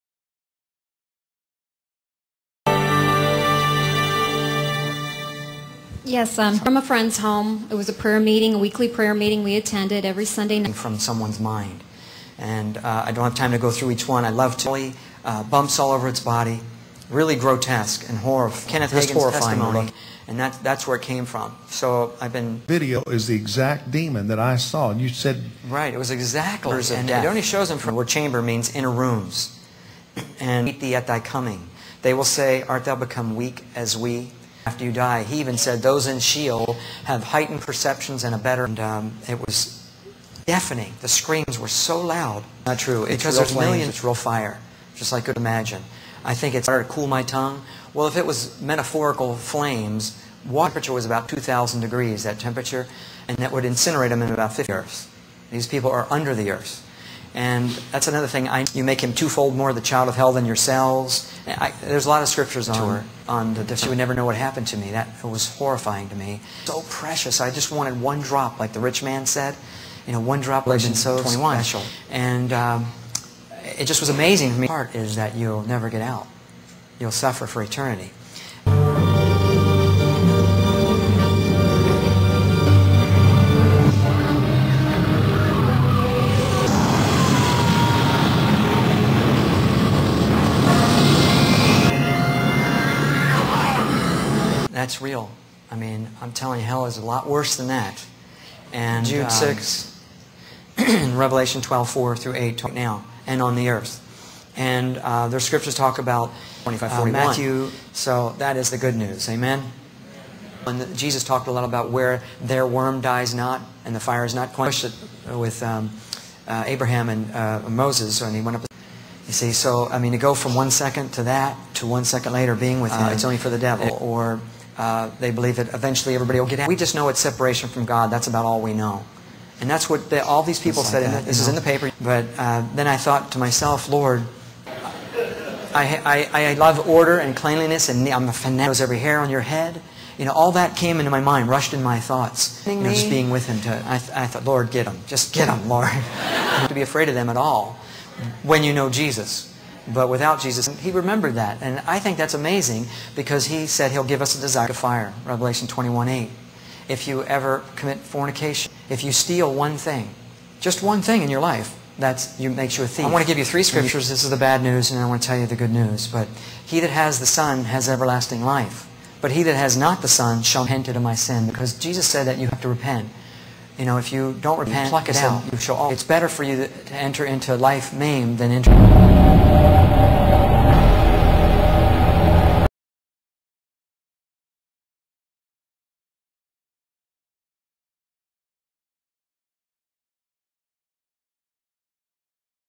23 Minutes in Hell
A man's personal experience of being taken out of body and transported to hell - then rescued by Jesus, so he can give this message of warning to the world.